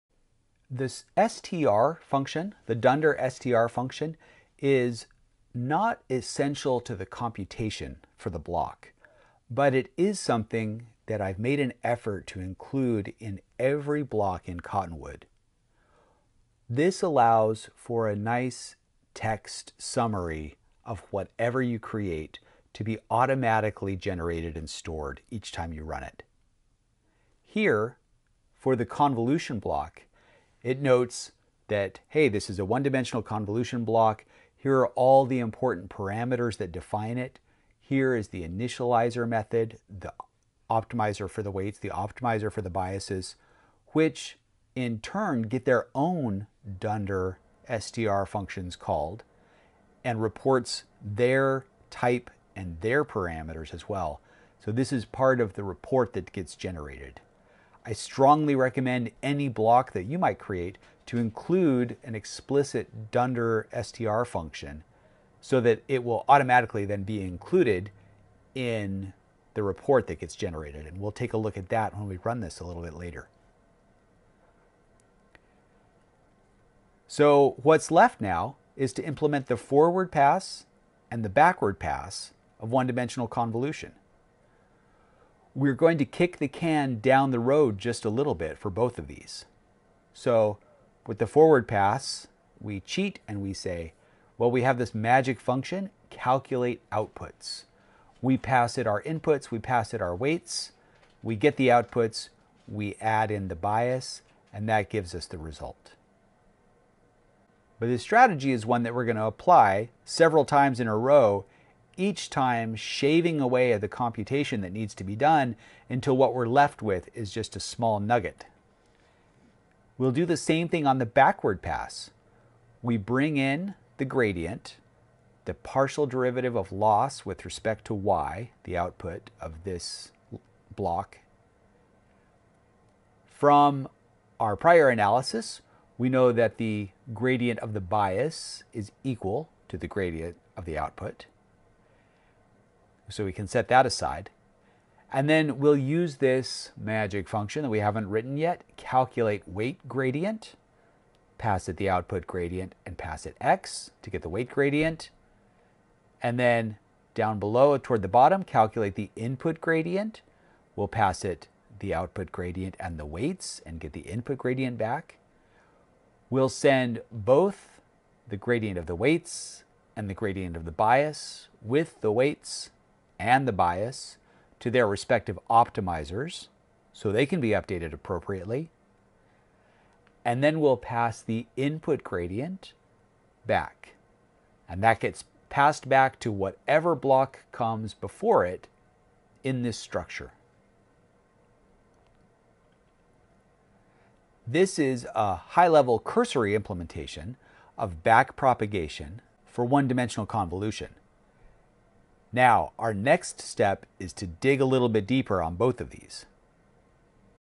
Implement 1D convolution, part 5: Forward and backward pass
This course starts out with all the fundamentals of convolutional neural networks in one dimension for maximum clarity. We will extend Cottonwood to handle convolutional architectures and apply it to classifying electrically-measured heartbeats as healthy or irregular.